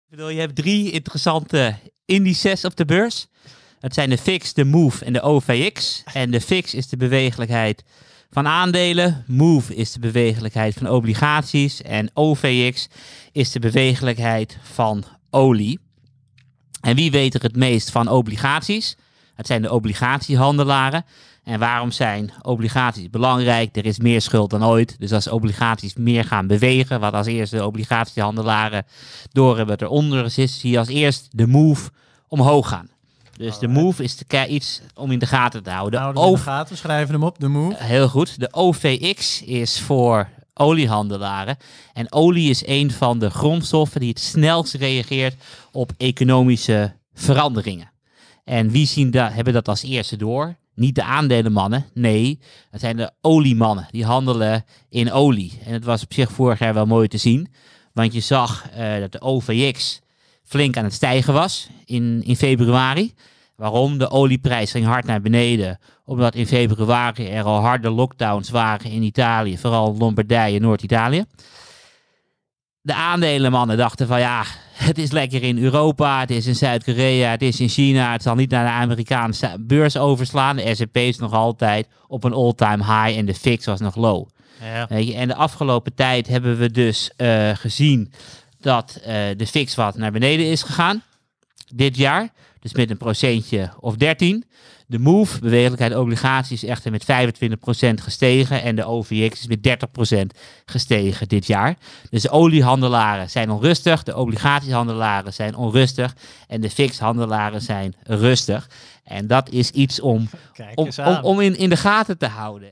Bewegelijkheid | Drie indices met een eigen verhaal
Karel Mercx over (on)rust bij olie-, obligatie- en aandelenhandelaren

In de tiende aflevering van Voorkennis praten we beleggers bij over 3D-printen, nemen we een dalende opkomende markten-ETF onder de loep, zetten we twee sectorgenoten tegen elkaar af en hoe nu verder na een jaar lang bullmarkt | Luister hier verder